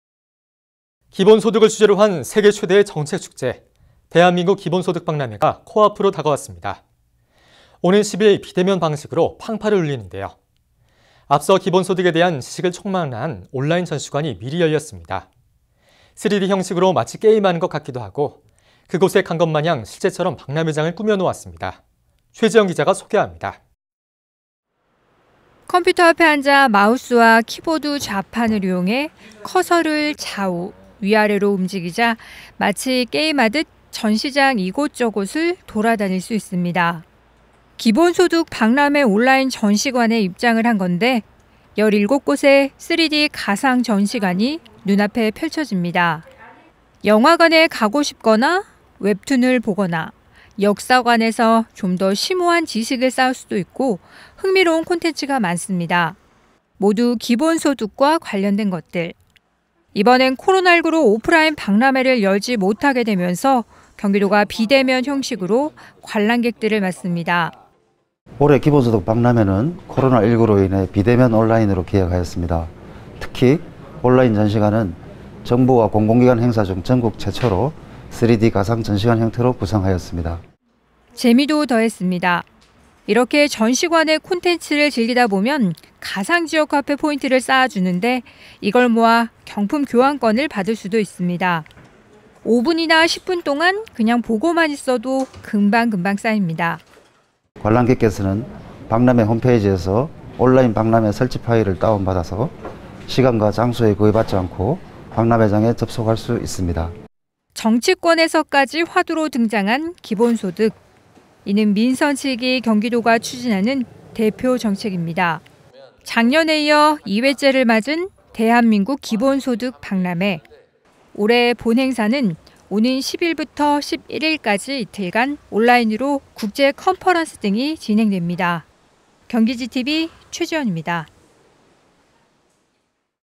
미리 보는 기본소득 박람회 게임 같은 온라인관
[앵커멘트]
‘기본소득’을 주제로 한 세계 최대의 정책축제 ‘대한민국 기본소득 박람회’가 코앞으로 다가왔습니다. 오는 10일 비대면 방식으로 팡파르를 울리는데요. 앞서 기본소득에 대한 지식을 총망라한 온라인 전시관이 미리 열렸습니다. 3D 형식으로 마치 게임하는 것 같기도 하고, 그곳에 간 것 마냥 실제처럼 박람회장을 꾸며놓았습니다. 최지현 기자가 소개합니다.

[리포트]
컴퓨터 앞에 앉아 마우스와 키보드 좌판을 이용해 커서를 좌우, 위아래로 움직이자 마치 게임하듯 전시장 이곳저곳을 돌아다닐 수 있습니다.

‘기본소득 박람회’ 온라인 전시관에 입장을 한 건데, 17곳의 3D 가상 전시관이 눈앞에 펼쳐집니다.

영화관에 가고 싶거나 웹툰을 보거나 역사관에서 좀 더 심오한 지식을 쌓을 수도 있고 흥미로운 콘텐츠가 많습니다.

모두 기본소득과 관련된 것들.

이번엔 코로나19로 오프라인 박람회를 열지 못하게 되면서 경기도가 비대면 형식으로 관람객들을 맞습니다.
[인터뷰]김재용/ 경기도 정책공약수석
“올해 기본소득 박람회는 코로나19로 인해 비대면 온라인으로 기획했습니다. 특히 온라인 전시관은 정부와 공공기관 행사 중 전국 최초로 3D 가상 전시관 형태로 구성했습니다.”

재미도 더했습니다.
이렇게 전시관의 콘텐츠를 즐기다 보면 가상지역화폐 포인트를 쌓아주는데, 이걸 모아 경품교환권을 받을 수도 있습니다.
5분이나 10분 동안 그냥 보고만 있어도 금방 금방 쌓입니다.

“관람객께서는 박람회 홈페이지에서 온라인 박람회 설치 파일을 다운로드 해 시간과 장소에 구애 받지 않고 박람회장에 접속할 수 있습니다.”

정치권에서까지 화두로 등장한 ‘기본소득’.
이는 민선 7기 경기도가 추진하는 대표 정책입니다.

작년에 이어 2회째를 맞은 ‘대한민국 기본소득 박람회’.
올해 본 행사는 오는 10일부터 11일까지, 이틀간 온라인으로 국제콘퍼런스 등이 진행됩니다. 경기GTV 최지현입니다.

영상취재: 이효진, 영상편집: 김상환

[자막]
1. 개막 앞두고 ‘온라인 전시관’ 오픈
2.박람회 홈페이지(basicincomefair.gg.go.kr)에 개관
3. 17개 가상 3D 전시관 투어하며 관람 가능
4. 본 행사 10일 비대면으로 온라인에서 팡파르
5. [인터뷰]김재용/ 경기도 정책공약수석
6. 가상 경기지역화폐 획득 이벤트 등 재미 요소도 가득
7. [인터뷰]김재용/ 경기도 정책공약수석